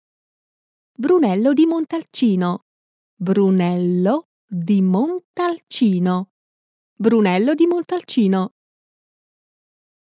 Brunello di Montalcino
Brunello di Montalcino is a red Italian wine produced in the vineyards surrounding the town of Montalcino located not so far from Florence in Tuscany.